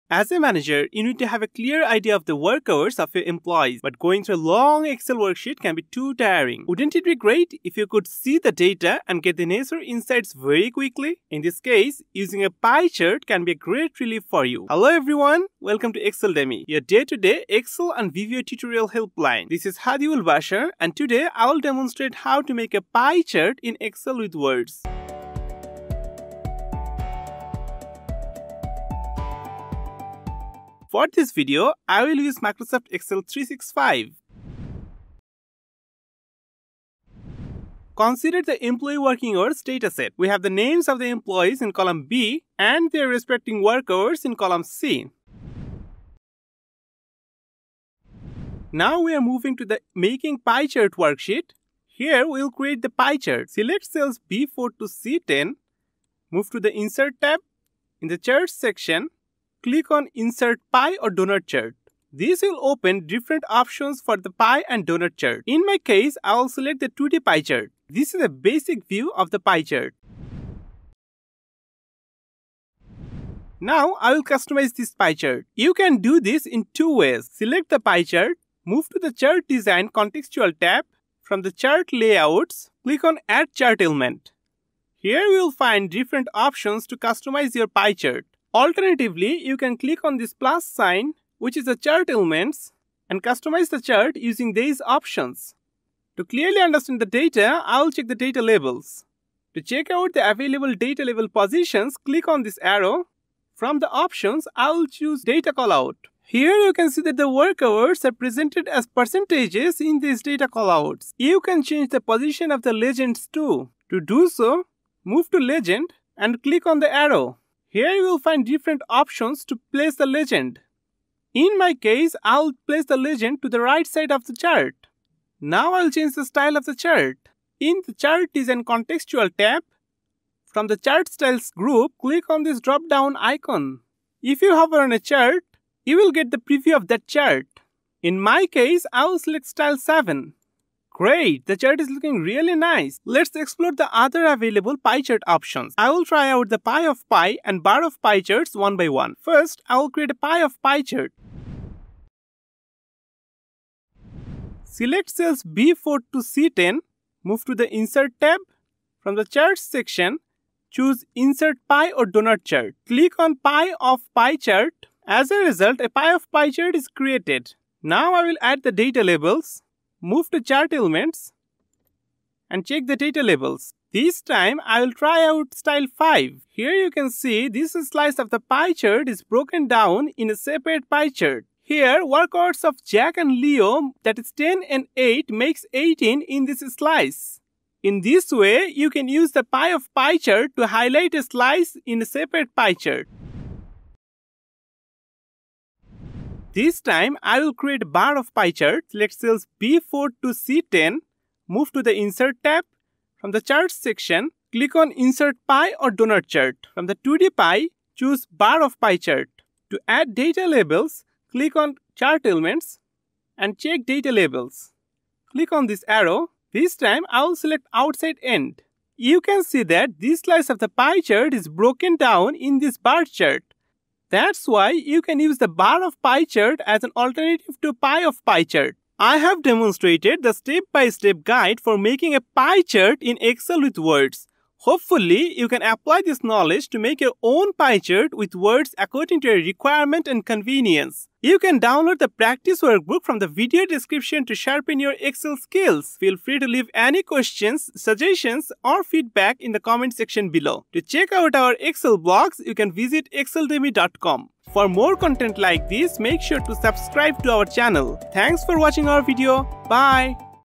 How to Make a Pie Chart in Excel with Words
In this video, I'll guide you through multiple steps to make a pie chart with words in Excel. You'll learn about arranging datasets, making Excel pie charts with words, labeling chart data, creating a pie of pie chart, and creating a bar pie chart. Pie charts can be used to highlight trends and patterns in data in a way that is easy to understand. With practical examples and step-by-step instructions, you can create a pie chart with Words in your own Excel spreadsheets effortlessly.

👨‍🏫 Instructor: Hadi Ul Bashar
🎥 Editor: Prantik Chowdhury

✨ ⯆ Resources:
Ctrl+B - To make text bold

0:00 - Intro
0:37 - Arranging dataset for making a pie chart
0:48 - Making Excel pie chart with words
1:13 - Labeling chart data
2:34 - Creating a pie of pie chart
3:16 - Creating a bar of pie chart